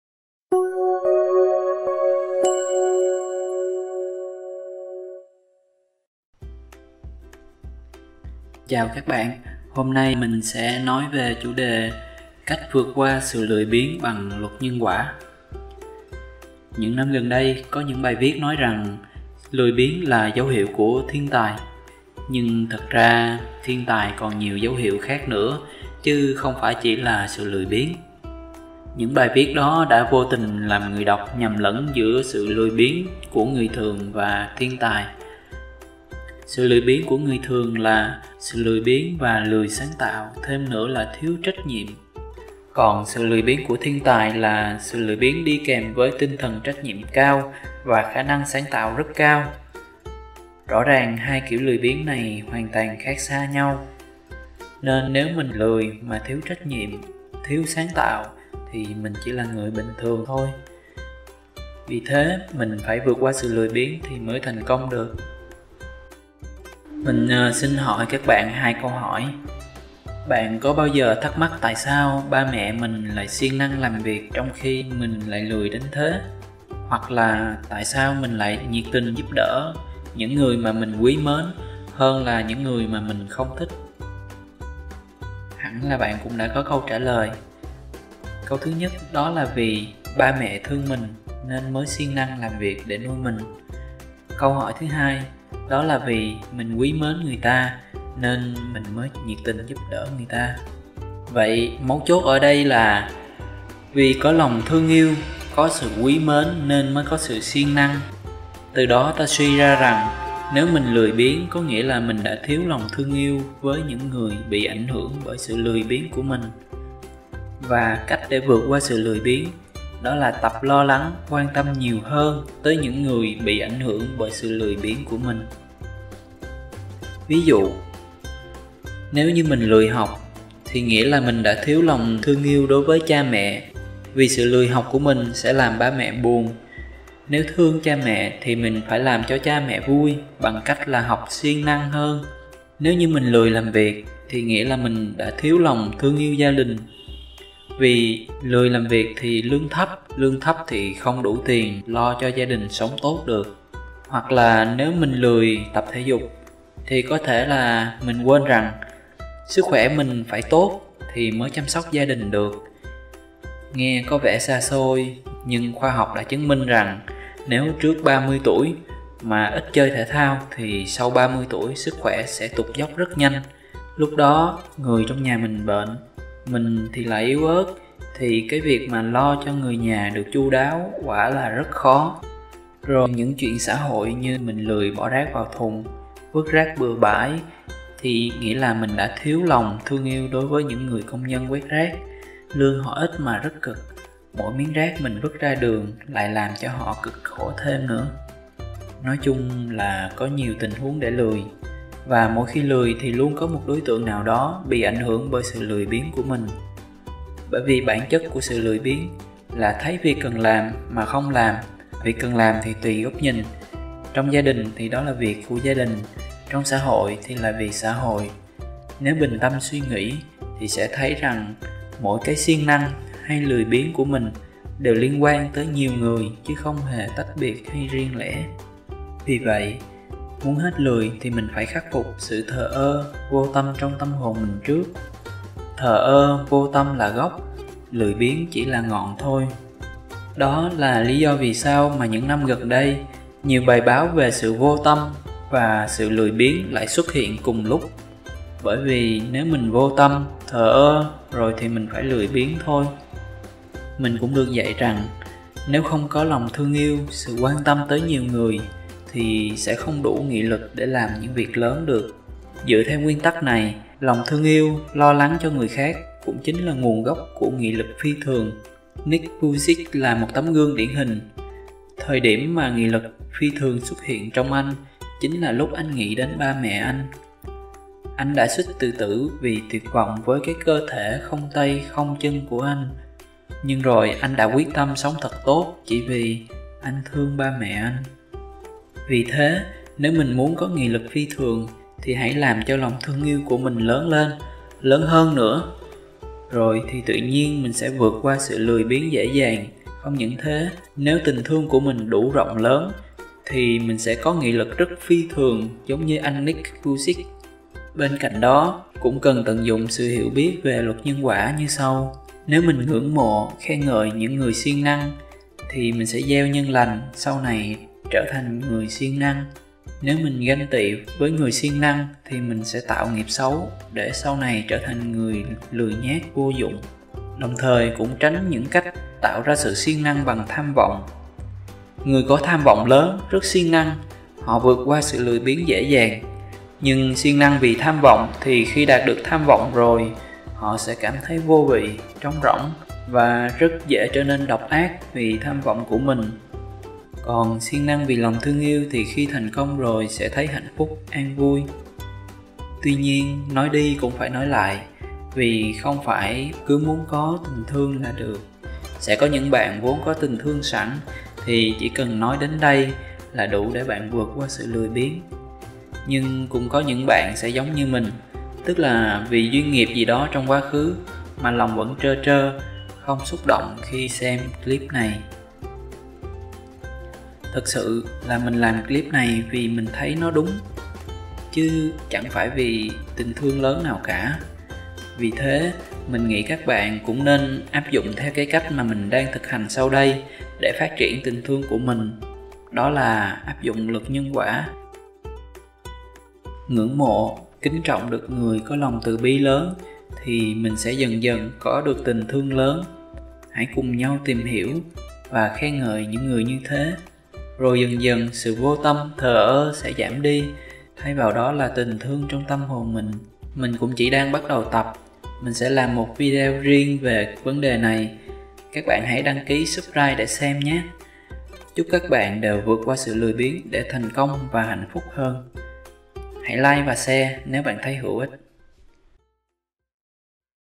Cách vượt qua sự lười biếng | phần 1 | HatBuiNho
Làm sao để vượt qua sự lười biếng để thành công và hạnh phúc hơn? Hãy xem clip để biết cách vượt qua ạ.

Những năm gần đây có những bài viết nói rằng lười biếng là dấu hiệu của thiên tài. Nhưng thật ra thiên tài còn nhiều dấu hiệu khác nữa, chứ không phải chỉ là lười biếng.

Cũng vì vậy mà có sự nhầm lẫn giữ sự lười biếng của người thường và thiên tài.

Sự lười biếng của người thường là: sự lười biếng và lười sáng tạo thêm nữa là thiếu trách nhiệm.

Còn sự lười biếng của thiên tài là: sự lười biếng và tinh thần trách nhiệm cùng với khả năng sáng tạo rất cao.

Nếu mình không phải là một thiên tài có trách nhiệm và sáng tạo, thì mình phải vượt qua sự lười biếng thì mới thành công được.

Bạn có bao giờ thắc mắc tại sao ba mẹ lại siêng năng làm việc trong khi mình thì lại lười đến thế?
Tại sao mình lại nhiệt tình giúp đỡ một người mà mình quý mến hơn là giúp đỡ một người mình ghét?

Hẳn bạn cũng đã có câu trả lời.
Câu hỏi 1: Đó là vì ba mẹ thương mình nên mới siêng làm việc để nuôi mình.

Câu hỏi 2: Là vì mình quý mến người ta nên mình mới nhiệt tình giúp người ta.

Vậy mấu chốt ở đâu là: vì có lòng thương yêu, có sự quý mến nên mới có sự siêng năng.

Từ đó suy ra rằng:
Nếu mình lười biếng có nghĩa là mình đã thiếu lòng thương yêu với những người bị ảnh hưởng bởi sự lười biếng của mình. Và cách để vượt qua sự lười biếng là tập lo lắng, quan tâm nhiều hơn tới những người bị ảnh hưởng bởi sự lười biếng của mình.

Vd:
Nếu mình lười học nghĩa là mình đã thiếu lòng thương yêu đối với cha mẹ. Vì sự lười học của mình sẽ làm ba mẹ buồn. Nếu thương cha mẹ thì phải làm cha mẹ vui bằng cách học siêng năng hơn.

Nếu mình lười làm việc nghĩa là mình đã thiếu lòng thương gia đình. Vì lười làm việc thì lương thấp, lương thấp không đủ tiền cho gia đình sống tốt được.

Nếu mình lười tập thể dục thì có thể mình quên rằng: sức khỏe mình phải tốt mới chăm sóc gia đình được. Nghe có vẻ xa xôi nhưng khoa học đã chứng minh nếu trước 30 tuổi mà ít chơi thể thao thì sau 30 tuổi sức khỏe sẽ tụt dốc rất nhanh. Lúc đó mà người trong nhà bị bệnh, mình thì lại yếu ớt thì việc lo cho người nhà được chu đáo là rất khó...

Rồi nếu mình lười bỏ rác vào thùng, vứt rác bừa bãi thì nghĩa là mình đã thiếu lòng thương người công nhân quét rác, lương ít mà rất cực. Mỗi miếng rác mình vứt ra đường là làm họ cực khổ thêm.

Nói chung rất là nhiều tình huống để lười.

Và mỗi khi lười thì luôn có 1 đối tượng nào đó bị ảnh hưởng bởi sự lười biếng của mình. Bởi vì bản chất của sự lười biếng là thấy việc cần làm mà không làm. Và việc cần làm thì tùy góc nhìn. Trong gia đình thì việc gia đình, trong xã hội thì việc xã hội. Nếu bình tâm suy nghĩ thì sẽ thấy mỗi cái siêng năng hay lười biếng của mình liên quan tới nhiều người chứ không hề tách biệt...

Đây là điều mình đã rút ra khi nghe Thầy mình dạy rằng:
Nếu không có lòng thương, sự quan tâm tới nhiều người thì sẽ không có siêng năng để làm việc lớn vì cộng đồng.

Vì vậy, muốn hết lười thì mình phải khắc phục sự thờ ơ, vô tâm trong tâm hồn mình trước.

Thờ ơ, vô tâm là gốc, lười biếng chỉ là ngọn.

Đó cũng là lý do vì sao mà những năm gần đây, nhiều bài báo đưa tin về sự vô tâm, xuất hiện cùng lúc với , những bài báo về sự lười biếng. Bởi vì mình vô tâm, thờ ơ rồi thì mình phải lười biếng.

Cũng dựa vào nguyên tắc này, sự lòng thương yêu, lo lắng cho người khác cũng là nguồn gốc của nghị lực phi thường.

Nick Vujicic là một tấm gương điển hình. Thời điểm mà nghị lực phi thường xuất hiện trong anh chính là lúc anh nghĩ đến ba mẹ anh. Anh đã suýt tự tử vì buồn cái cơ thể không tay không chân của anh. Nhưng rồi anh đã quyết tâm sống cho tốt chỉ vì thương ba mẹ anh.

Nếu vậy thì bạn cần áp dụng thêm luật nhân quả sau đây:
Mình ngưỡng mộ được Người có lòng từ bi lớn thì mình cũng dần dần có tình thương lớn.

Các bạn hãy tiếp xúc, khen ngợi những Người như thế rồi dần dần sự vô tâm, thờ ơ sẽ ra đi thay bằng tình thương lớn.

Chúc các bạn thương yêu nhiều hơn để vượt qua sự lười biếng và thành công hạnh phúc hơn.